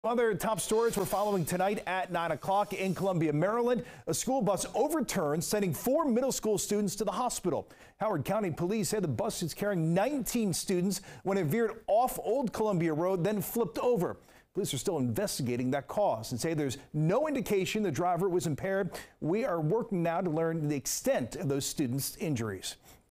School bus with children on board overturns in Columbia
The Howard County Police Department (HCPD) said officers were on the scene of an overturned school bus in Columbia Thursday afternoon.